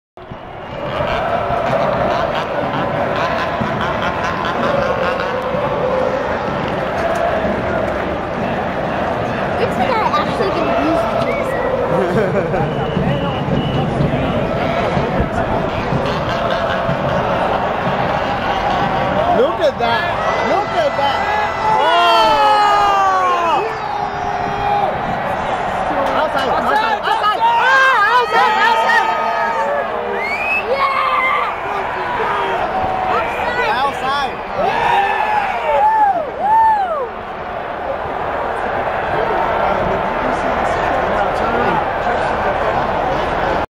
Timbers fans celebrate penalty
Timbers fans watching from outside PGE Park react to a Seattle goal, then the penalty that nullifies the goal Wednesday.